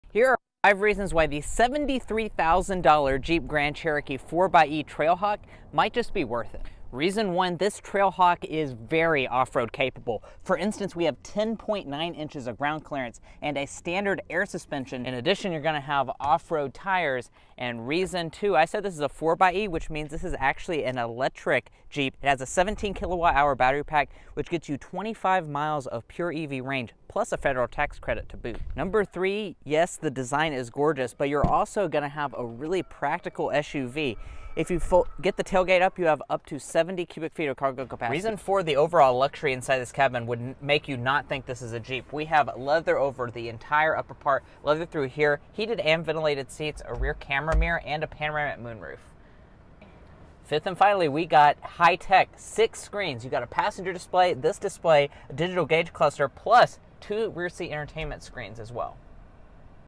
Jeep Grand Cherokee 4xe | Worth $73,000 for this plug-in hybrid?? 🔥👀
Here’s the Jeep Grand Cherokee 4Xe! Is it worth the money?